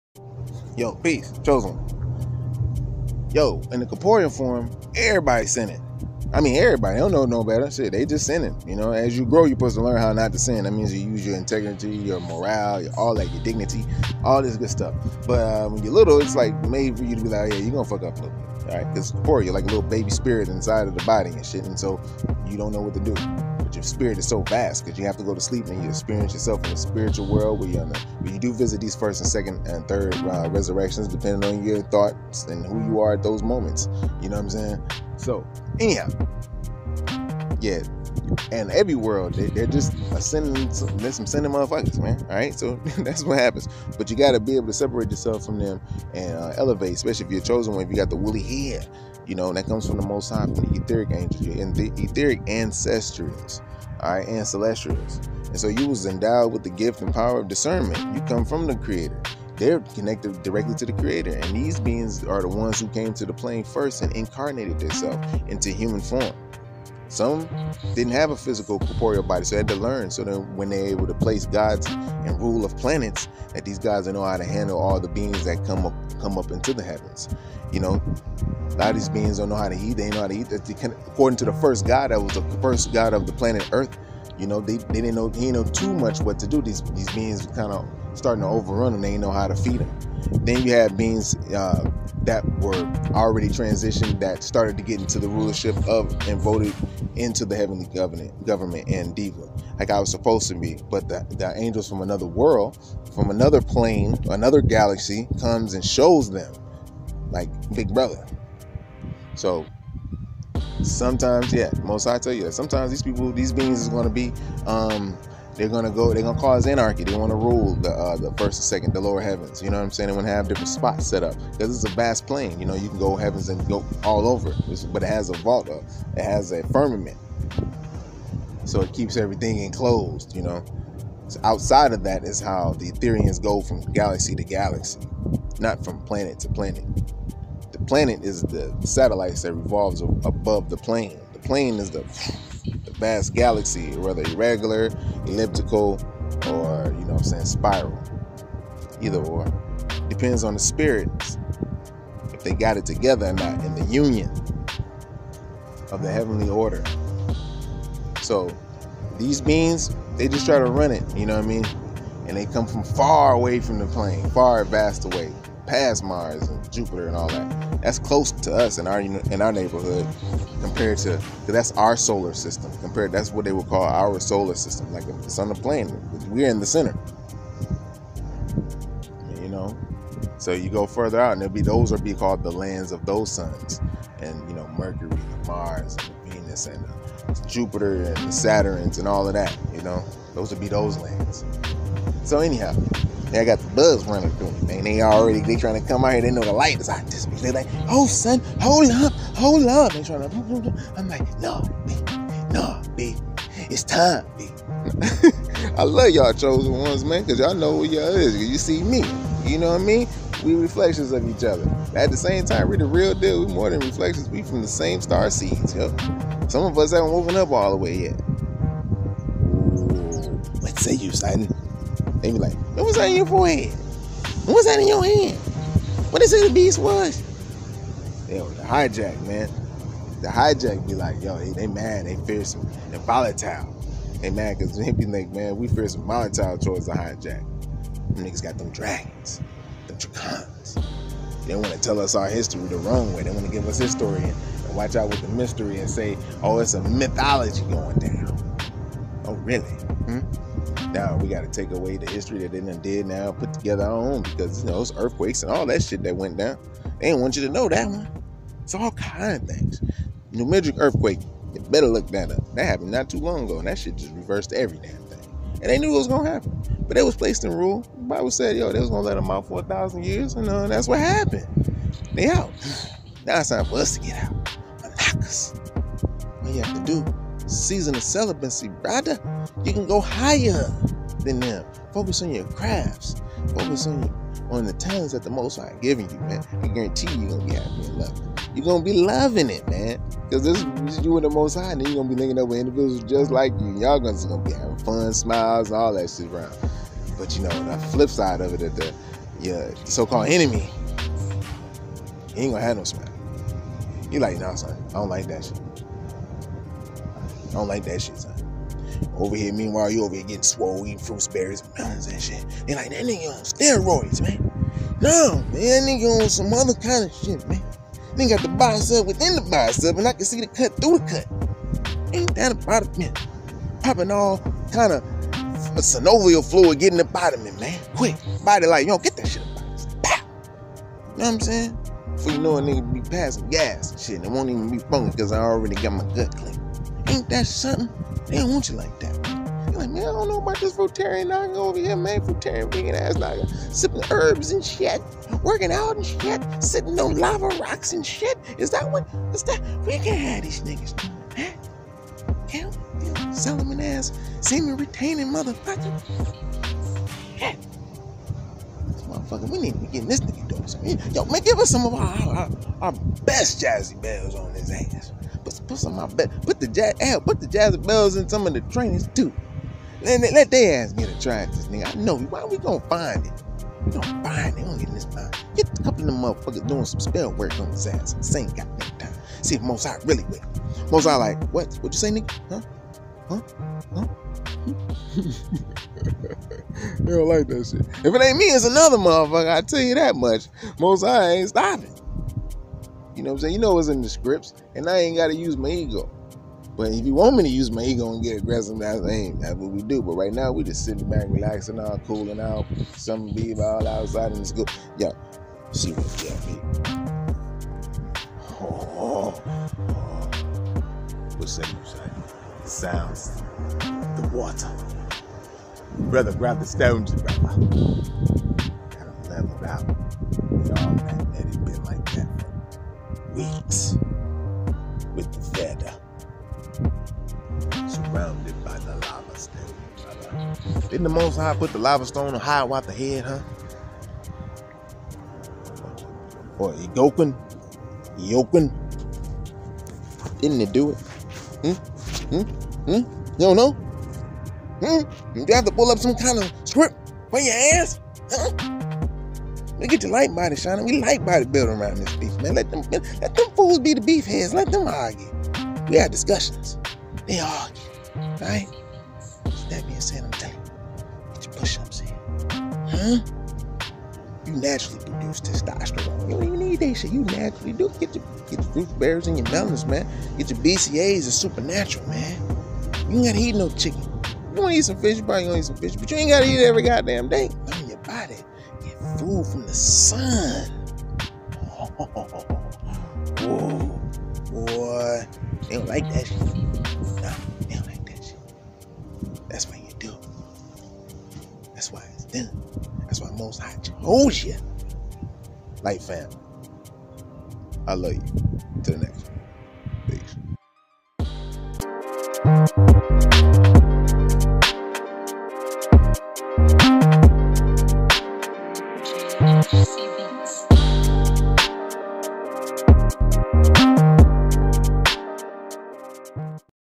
CHOSEN ONES* IN THE FLESH YOU LEARN HOW NOT TO SIN WHILE LEARNING HOW TO DIE* LIGHT BEEINGS
LIGHT BODY BULDING OFFGRID FRUITARIAN VEGAN MISSISSIPPI CHOCTAW NAGA DIVINE LIGHT BODY MEAL PLANS AVAILABLE NOW! Learn more about the cluster b personality disorder and how it was projected upon Amaru khans (black americans) and how it effects relationships as a whole